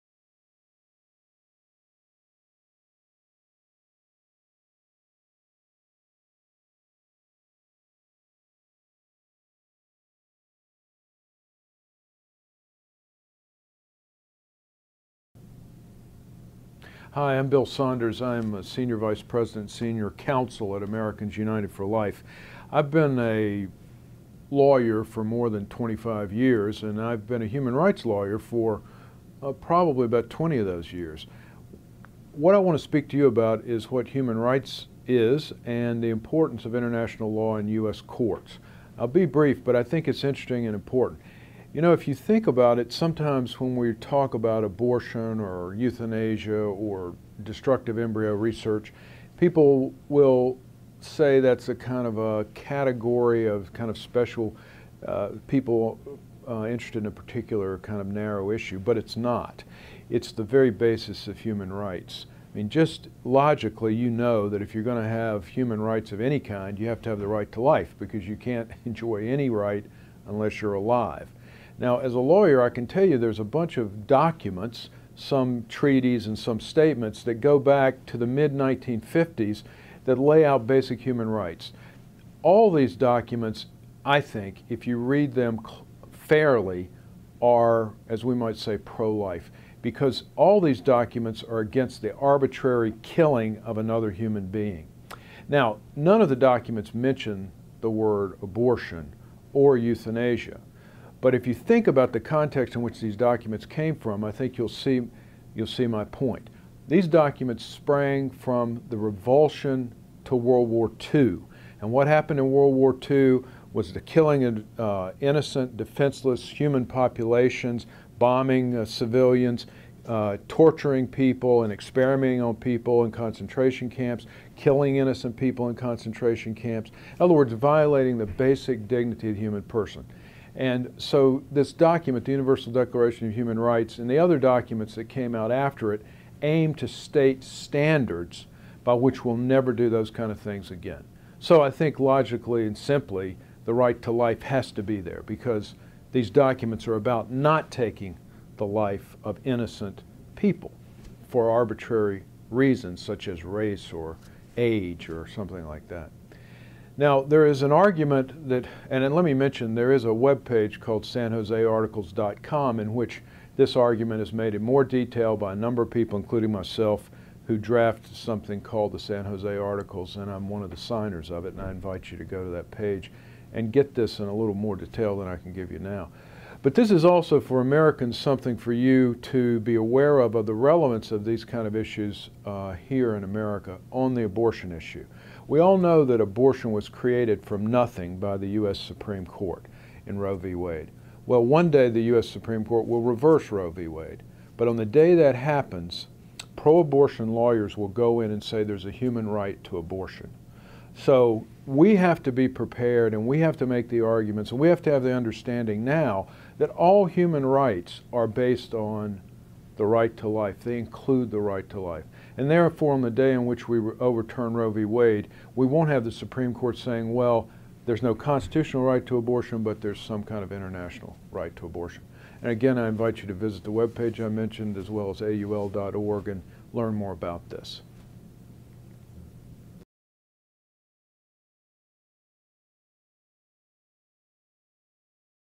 William Saunders - Human Rights and International Law
Bill Saunders of Americans United For Life discusses Human Rights and How International Law Affects US Courts. He discusses two important documents, the Universal Declaration of Human Rights and the San Jose Articles of which he is one of the signers of.